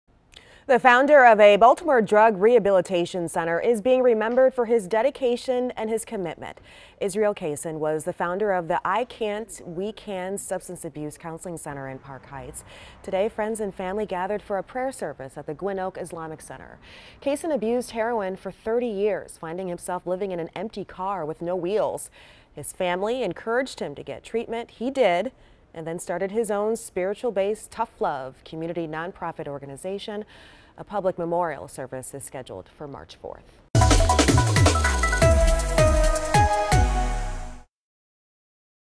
Baltimore drug rehabilitation center's founder dies at age 70
The founder of a Baltimore drug rehabilitation center is being remembered for his dedication and commitment. Israel Cason died at the age of 70. He was the founder of the "I Can't We Can" substance abuse counseling center in Park Heights. Friends and family gathered Thursday for a prayer service at the Gywnn Oak Islamic Center.